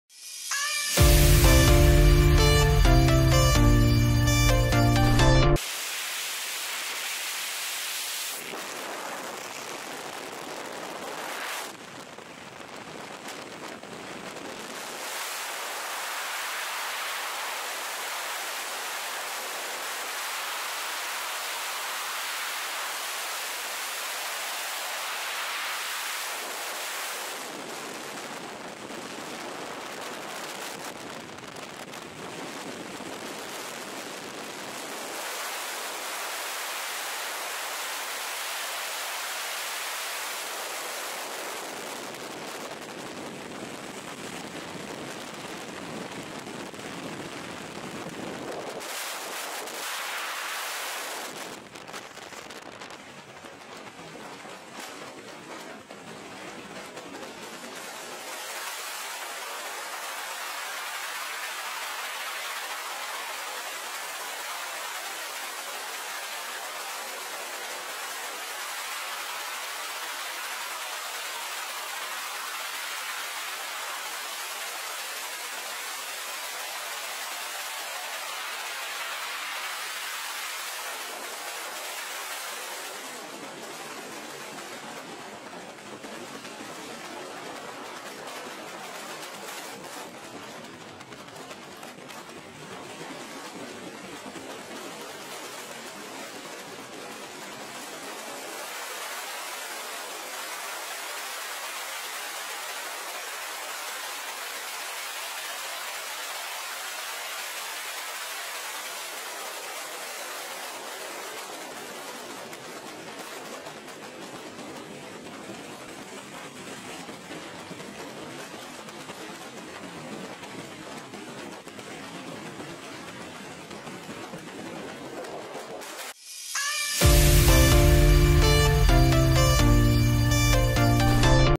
Friday Freakout: Skydiver Spins Out of Control
Pop Quiz: Is this 1) a skydiver losing control, 2) a "simulation" for a TV show or 3) a breakdance routine?